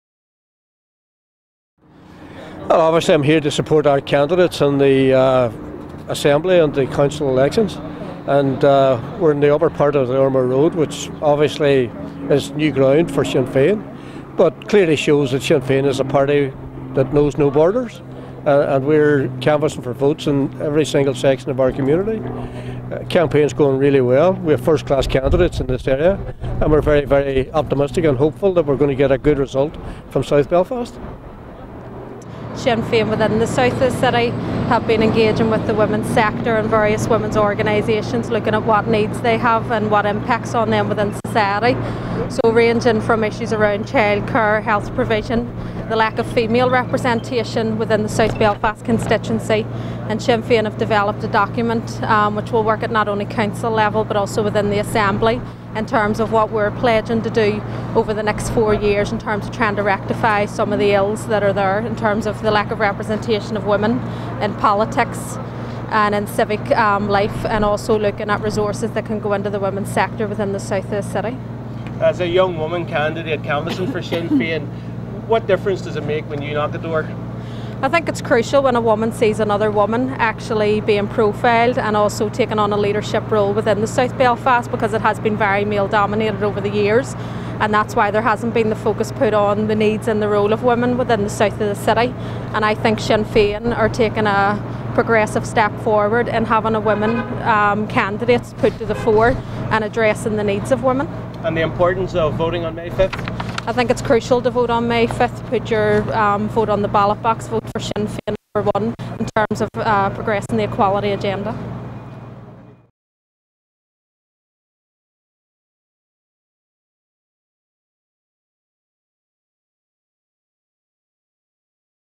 South Belfast Launch Billboard and Womens Document
Martin McGuinness breaks new ground for Sinn Fein by launching an election billboard on the Upper Ormeau Rd, while South Belfast Council Candidate Deirdre Hargey accompanied by Mary Lou McDonald TD, Bairbre de Brun MEP and Cllr. Toireasa Ferris launches a South Belfast Womens Document.